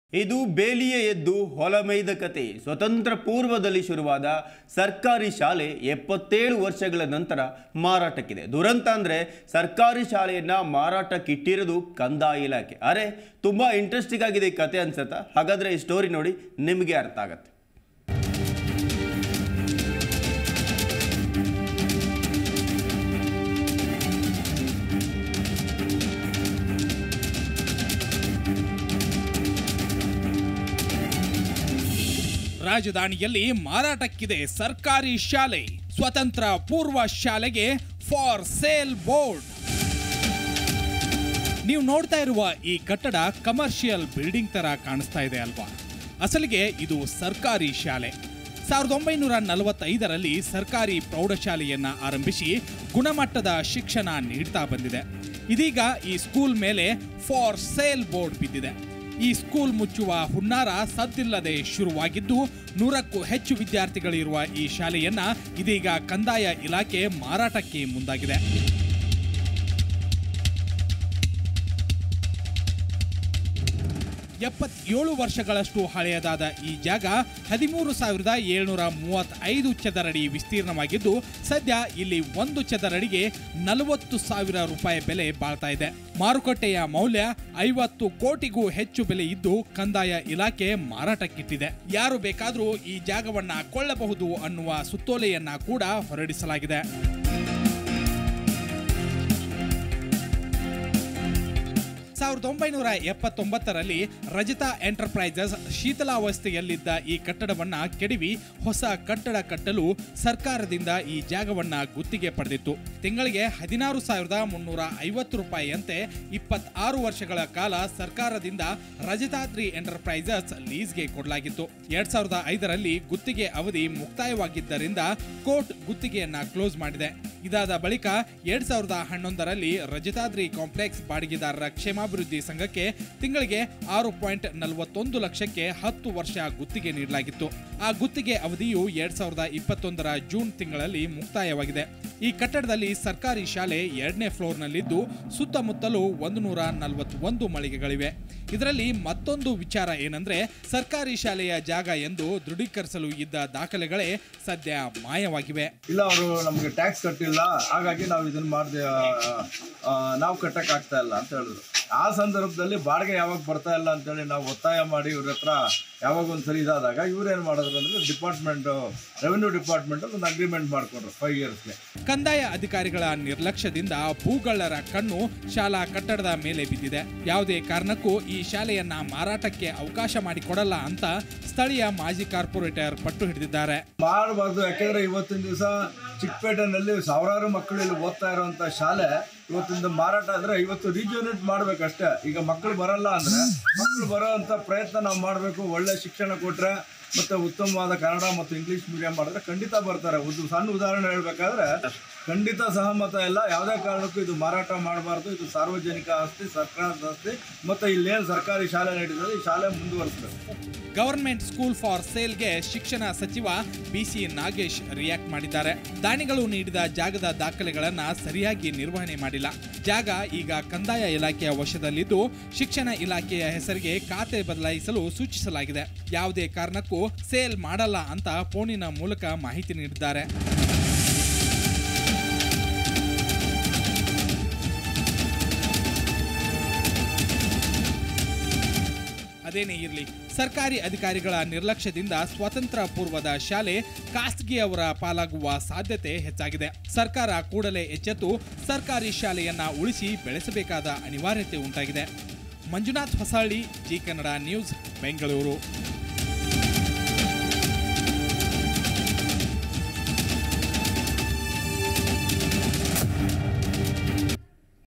SCHOOL FOR SALE || ಸ್ವಾತಂತ್ರ್ಯ ಪೂರ್ವ ಶಾಲೆಗೆ FOR SALE ಬೋರ್ಡ್‌
ಇದು ಬೇಲಿಯೇ ಎದ್ದು ಹೊಲ ಮೇಯ್ದ ಕಥೆ. ಸ್ವಾತಂತ್ರ್ಯ ಪೂರ್ವದಲ್ಲಿ ಶುರುವಾದ ಸರ್ಕಾರಿ ಶಾಲೆ 77 ವರ್ಷಗಳ ನಂತರ ಮಾರಾಟಕ್ಕಿದೆ. ದುರಂತ ಅಂದ್ರೆ ಸರ್ಕಾರಿ ಶಾಲೆಯನ್ನು ಮಾರಾಟಕ್ಕೆ ಇಟ್ಟಿರೋದು ಕಂದಾಯ ಇಲಾಖೆ. ಅರೇ, ಕಥೆ ತುಂಬಾ ಇಂಟ್ರೆಸ್ಟಿಂಗ್ ಅನ್ನಿಸುತ್ತಿದ್ಯಾ..? ಹಾಗಾದ್ರೆ ಈ ಸ್ಟೋರಿ ನೋಡಿ..